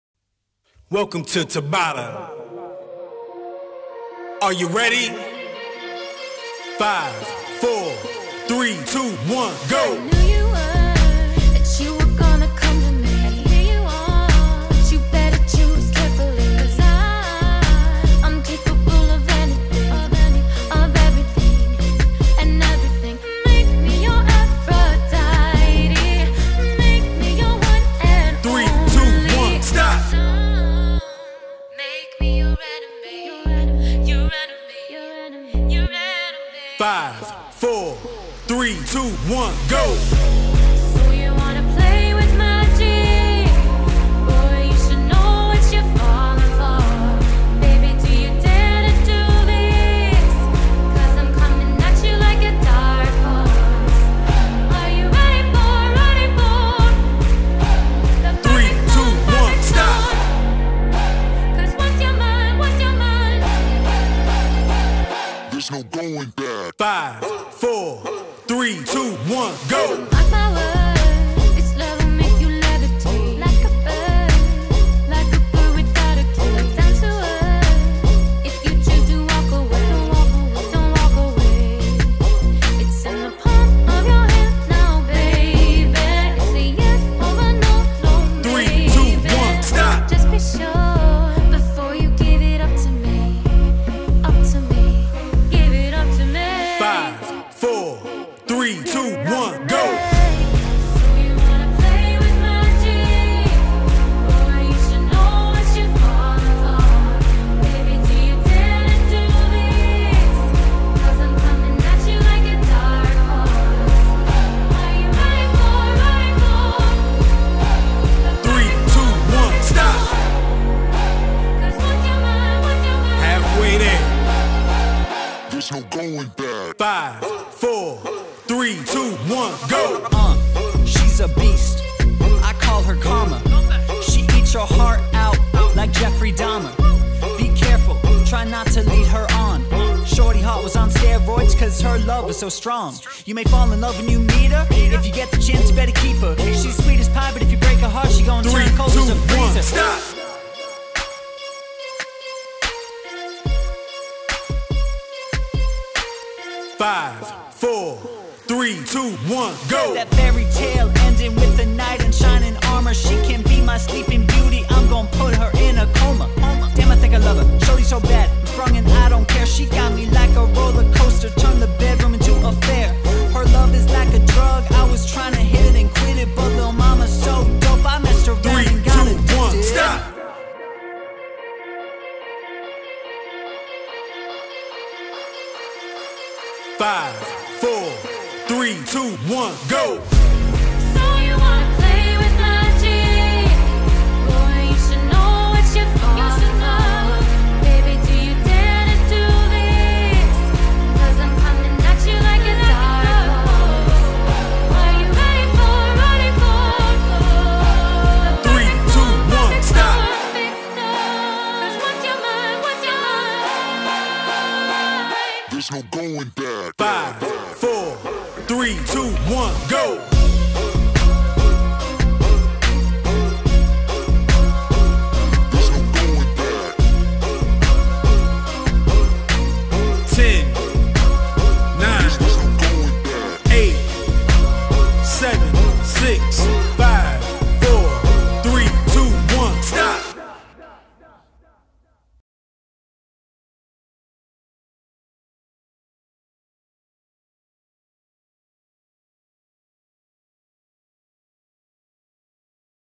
Fat Melting Tabata - Tribute to a Friend
KS is one of the founding members of my Tabata group which started almost 5 years ago. Here's a final Tabata session together with him before he embarks on his next journey in life